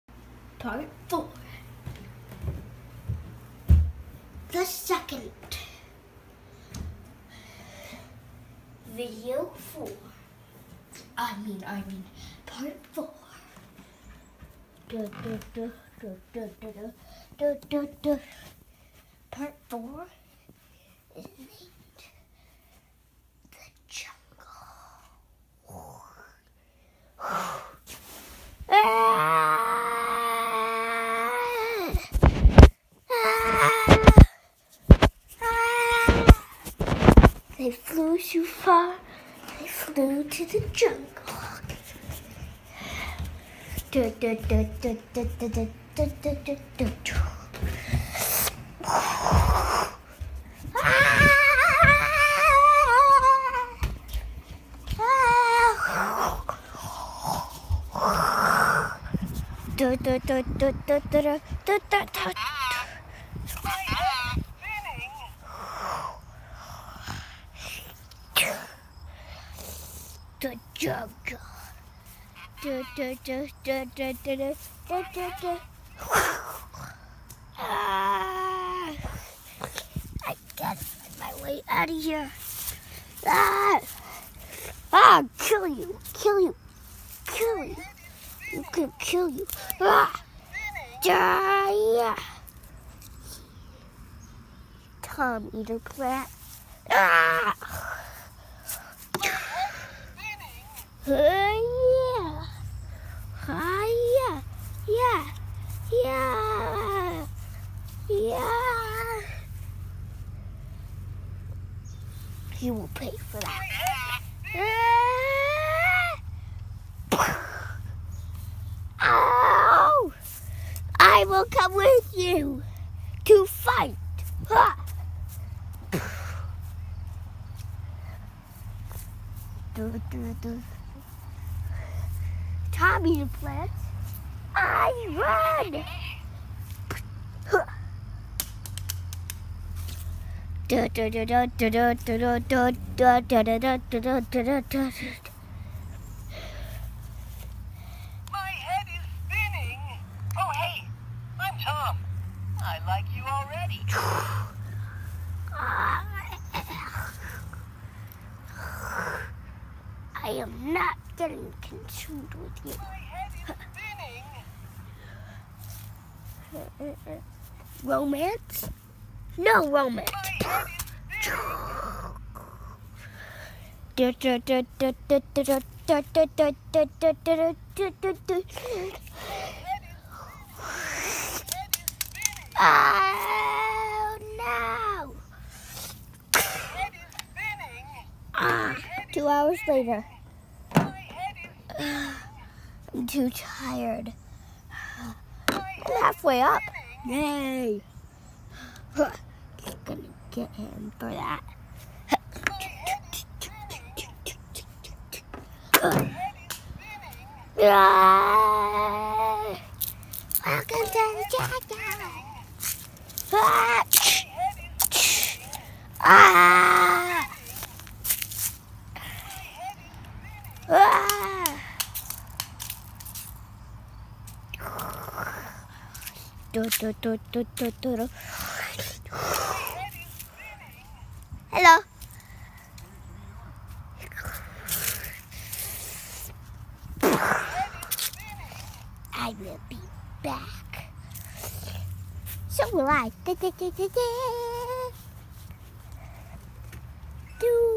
Part 4 The Jungle
Talking Toms are in the jungle hunting each other down.